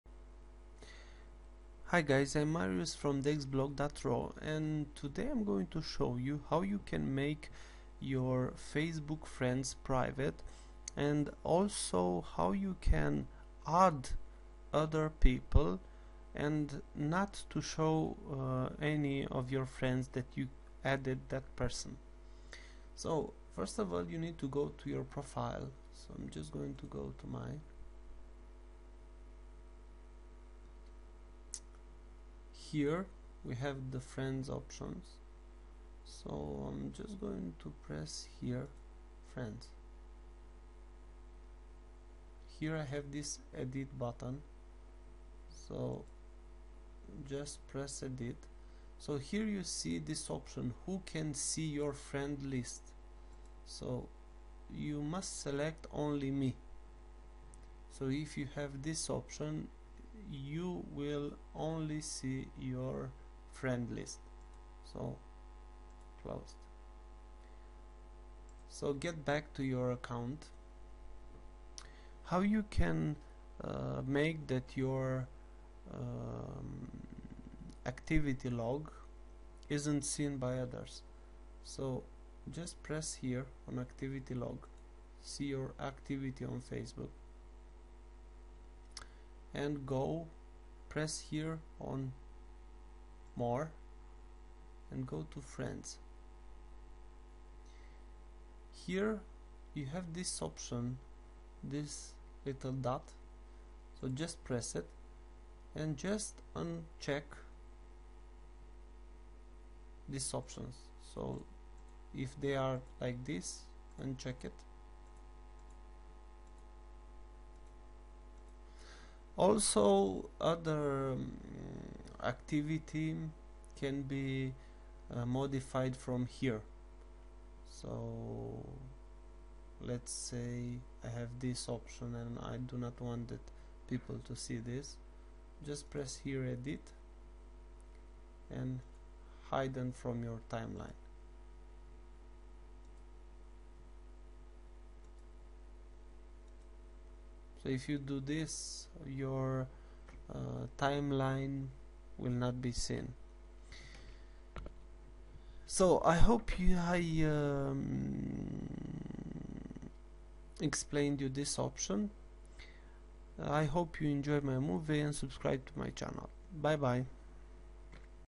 Maybe You Have Some Friends on Facebook
Maybe you have some friends on Facebook and you do not want that they see your friend list and also to see when you add a new friend.
This movie will show you how to delete from timeline this kind of data and also what to do so that your friends can not see who you add.